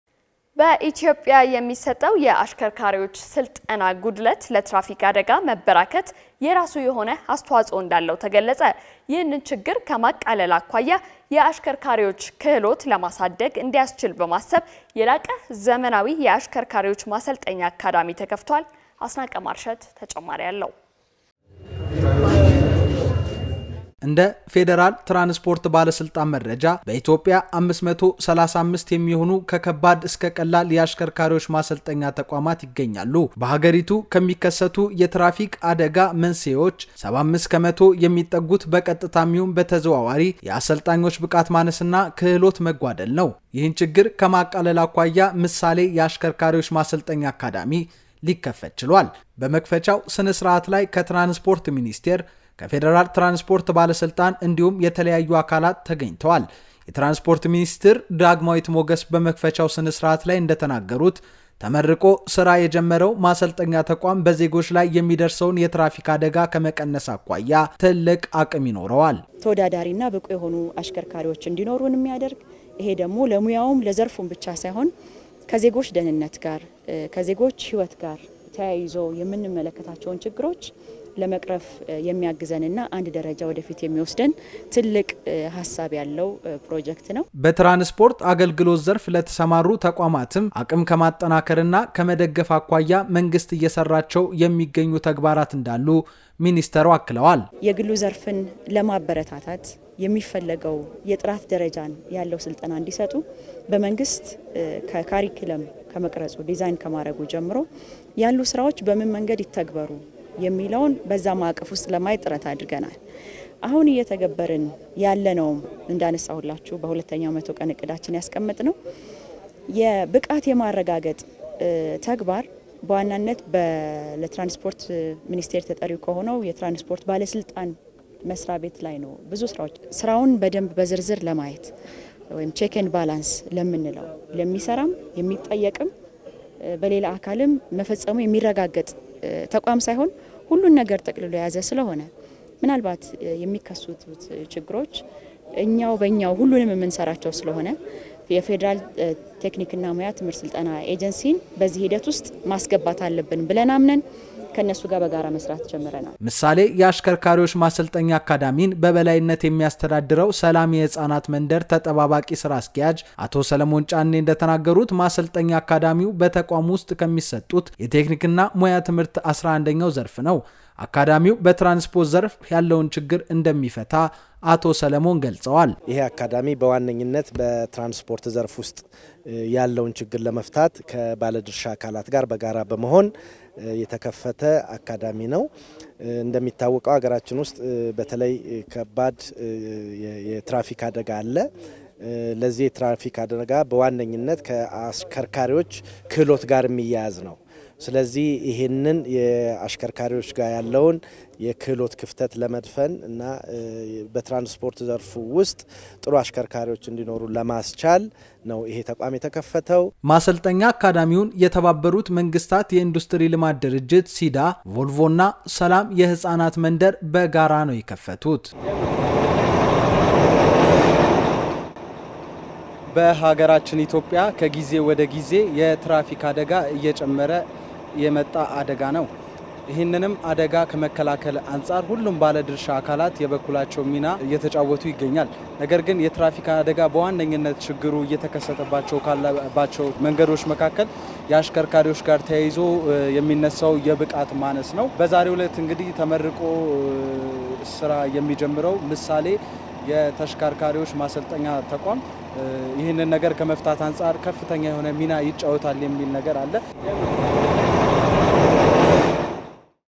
NAHOO NEWS- በኢትዮጵያ የሚሰጠው የአሽከርካሪዎች ስልጠና ጉድለት ለትራፊክ አደጋ መበራከት አስተዋፅኦ እንዳለው ተገለፀ፡፡NAHOO TV
በኢትዮጵያ የሚሰጠው የአሽከርካሪዎች ስልጠና ጉድለት ለትራፊክ አደጋ መበራከት  አስተዋፅኦ እንዳለው ተገለፀ፡፡